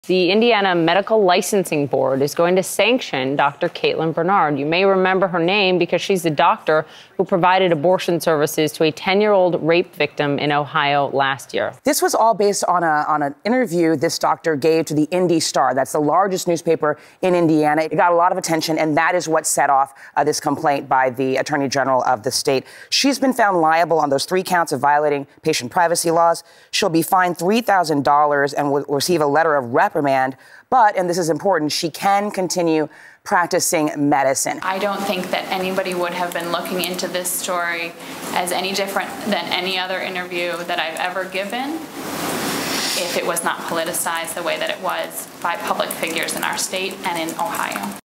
Indiana doctor who spoke about providing 10-year-old's abortion gets reprimanded
Dr. Caitlin Bernard, who provided an abortion to a 10-year-old has been fined by a medical board after speaking to media about the case.